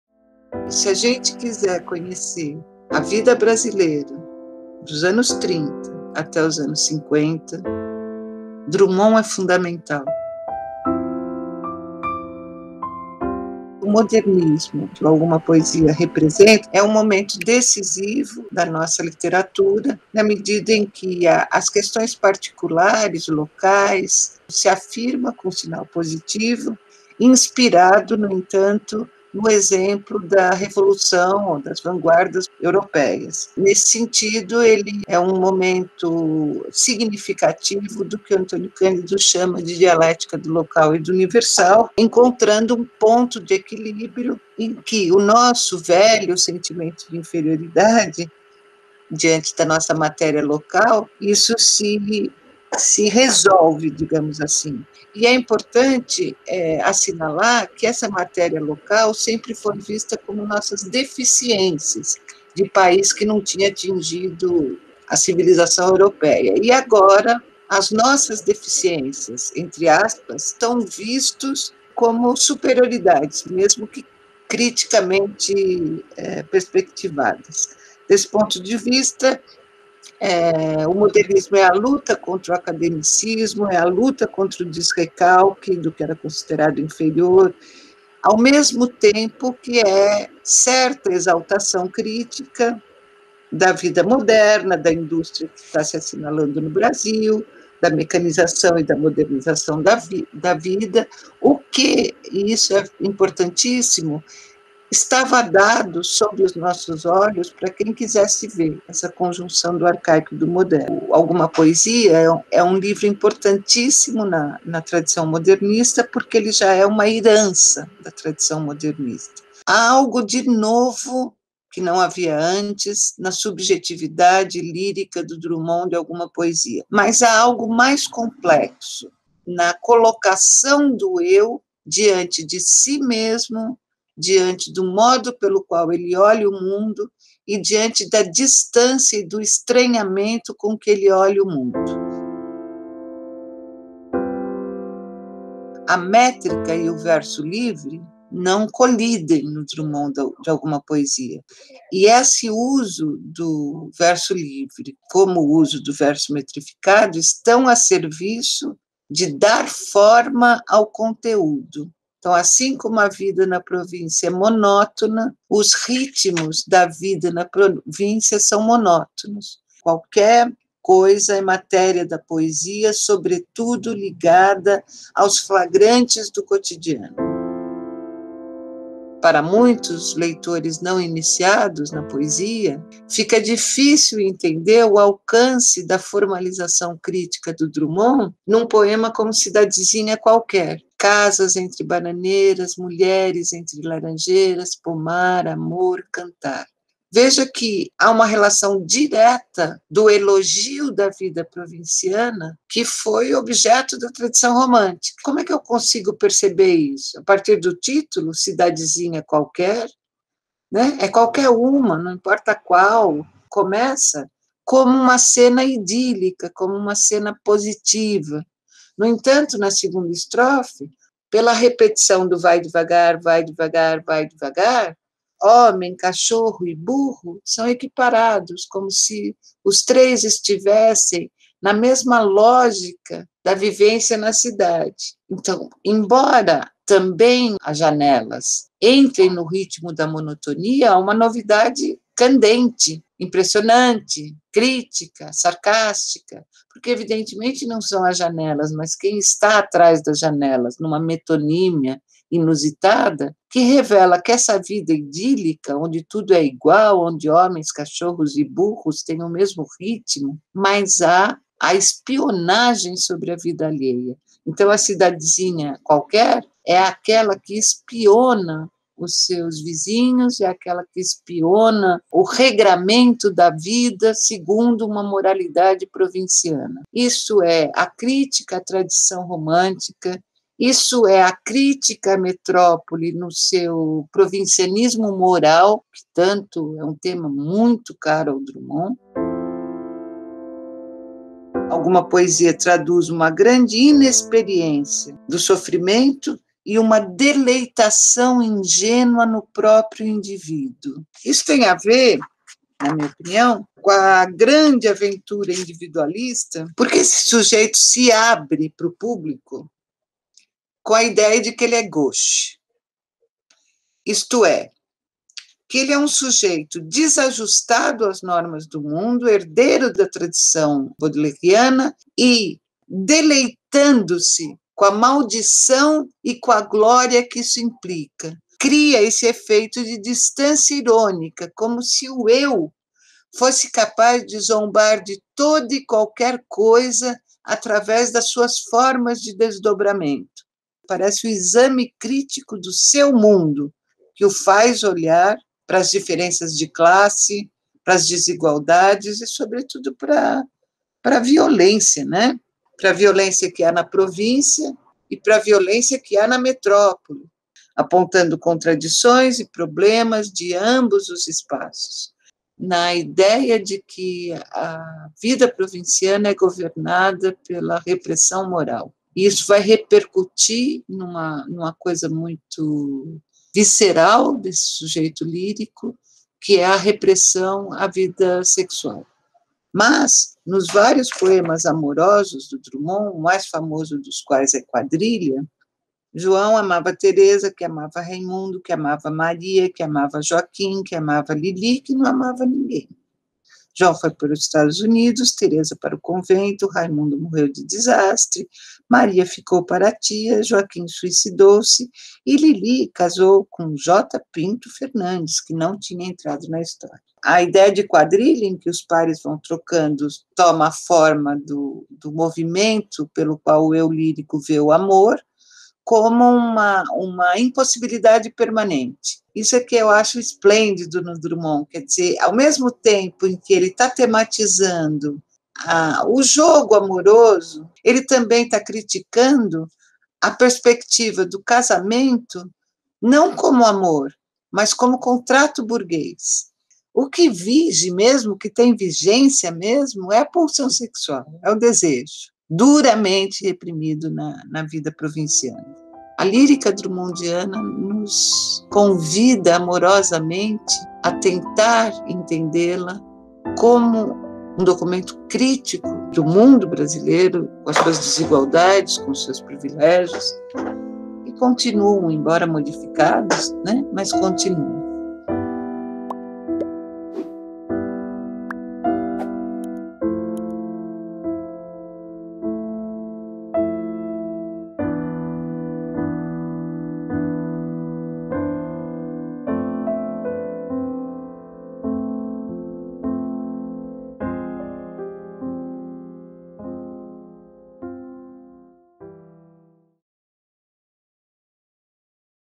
[Fuvest] Em “Alguma Poesia”, sujeito poético é chave para observar o Brasil
A professora Ivone Daré Rabello, da Faculdade de Filosofia, Letras e Ciências Humanas (FFLCH) da USP, analisa o livro "Alguma Poesia", de Carlos Drummond de Andrade, obra indicada para o vestibular da USP 2021, organizado pela Fuvest.